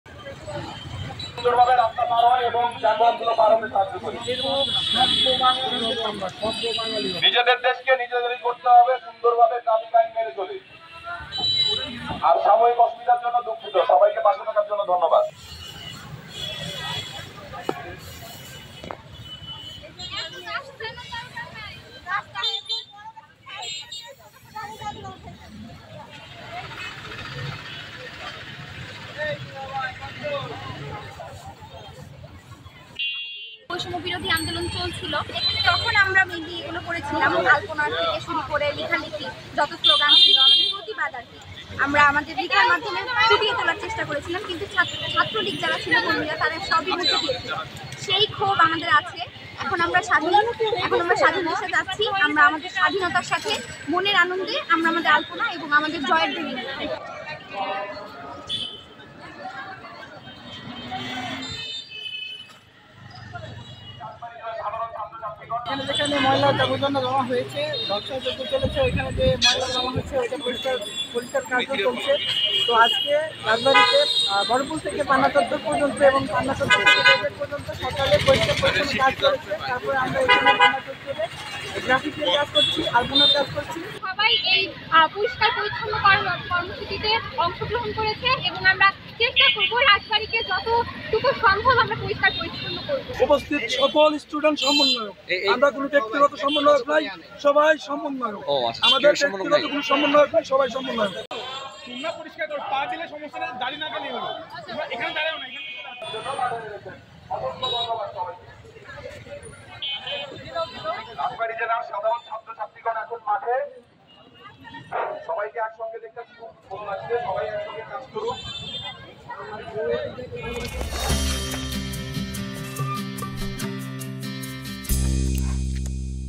রাজবাড়ীতে শিক্ষার্থীদের পরিষ্কার-পরিচ্ছন্নতা অভিযান | Rajbari News | Prothom Alo
বুধবার রাজবাড়ী শহরে পরিষ্কার–পরিচ্ছন্নতা অভিযান চালিয়েছেন শিক্ষার্থীরা। বিস্তারিত ভিডিওতে...